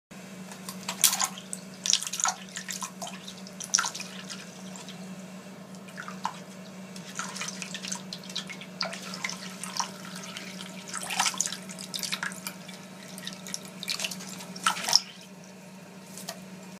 Tachikoma explores a drip of water
My ferret, Tachikoma, had been trying to get into my sink for some time, so I let her try it.